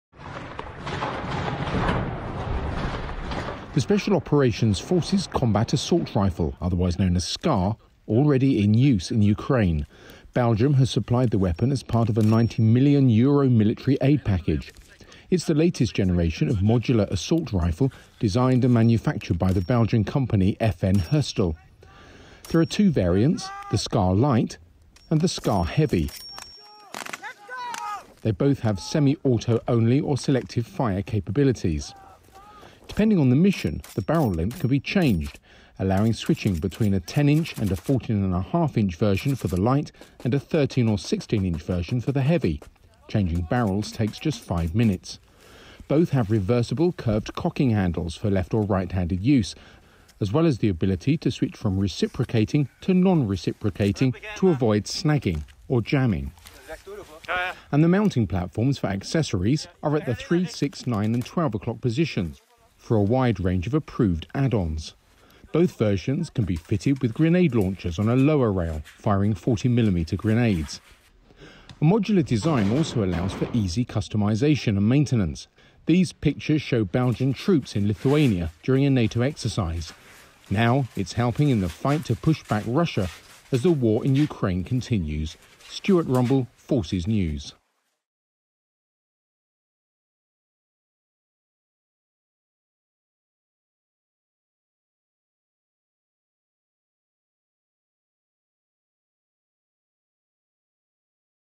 SCAR: What is the modular assault rifle being used by Ukrainian troops?
The Special Operations Forces combat assault rifle, otherwise known as SCAR, has been supplied to Ukraine by Belgium as part of a €90m military aid package.

It's the latest generation of modular assault rifles, designed and manufactured by the Belgian company FN Herstal.

As the war in Ukraine continues, the rifle is helping with the pushback of Russian forces.